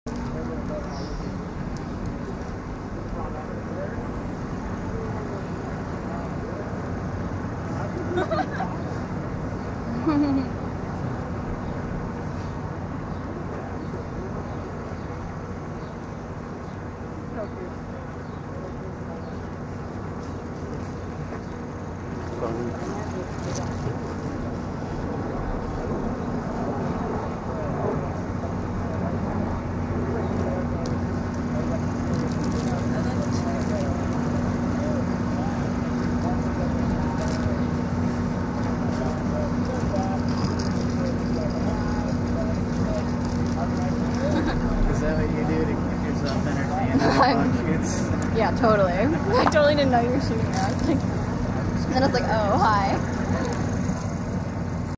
Gossip Girl - Taylor Momsen Having Fun
Taylor playing backstage in a photoshoot.